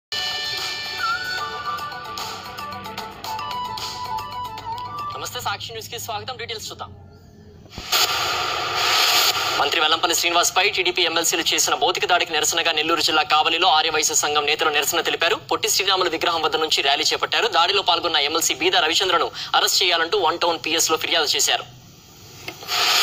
Kavali lo TDP MLC అరెస్ట్ చేయాలంటూ ఆర్యవైశ్య సంఘం నాయకులు ఆందోళన
Video from Kumar